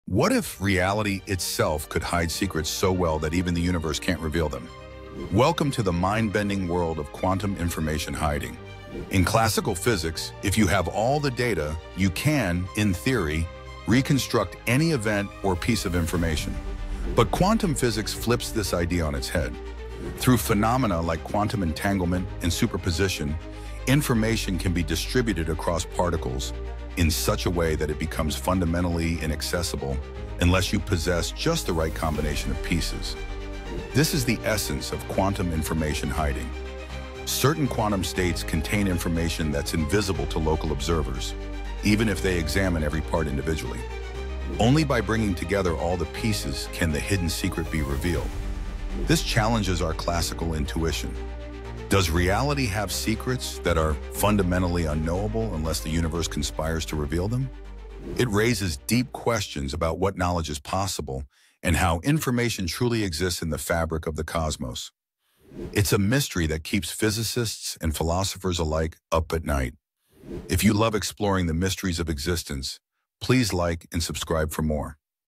Quantum Secrets: Can Reality Hide Information?
Can reality truly hide secrets within quantum states? Discover how quantum information hiding challenges our understanding of the universe!